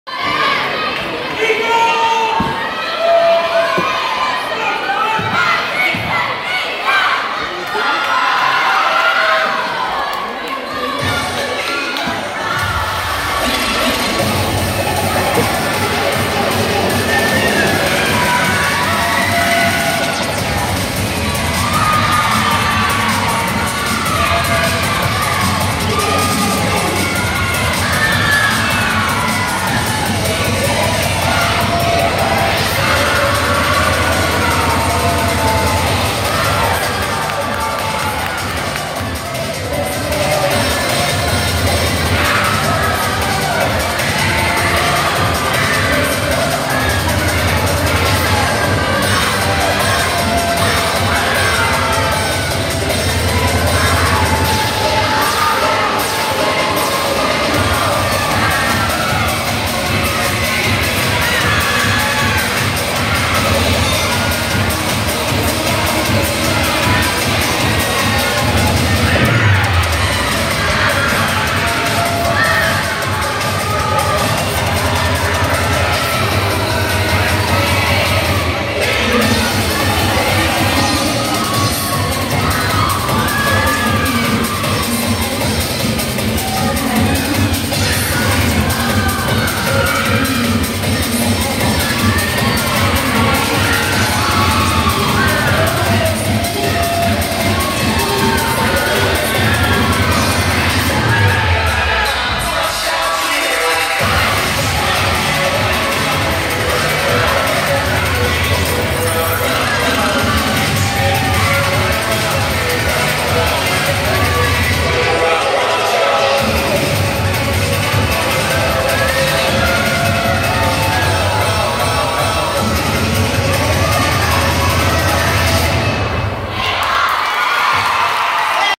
Santa Margarita Catholic HS Song/Pom (Varsity) @ 2018 USA Regionals
Santa Margarita Catholic High School
Varsity Song/Pom Advanced

United Spirit Association Regionals
University High School
Irvine, California